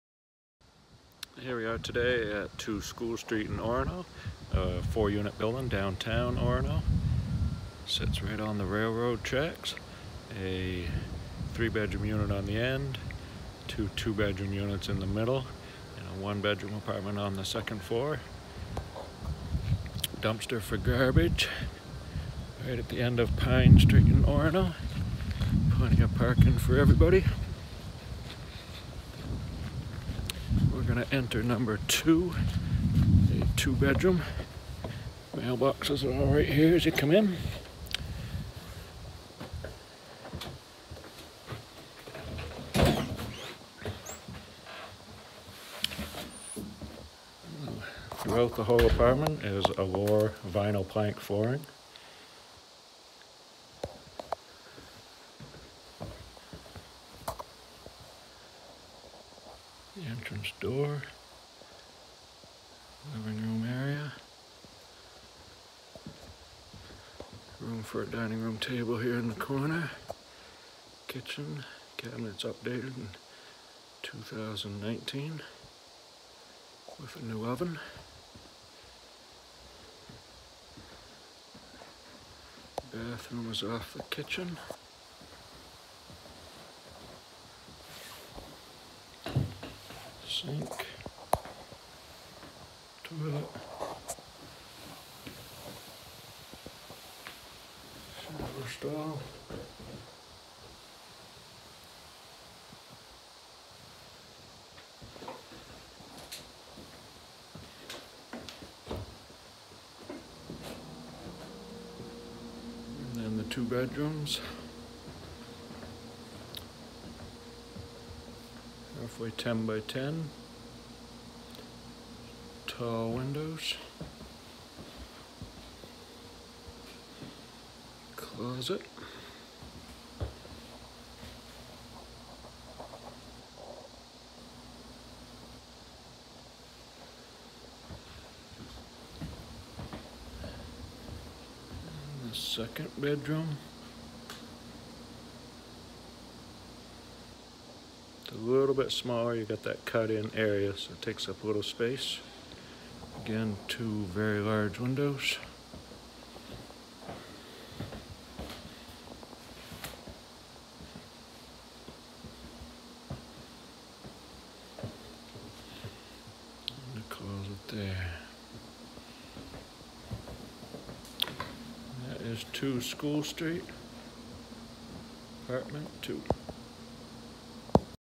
2 School Street #2, Orono 2020
2 bedroom apartment in downtown Orono. A 4 unit building.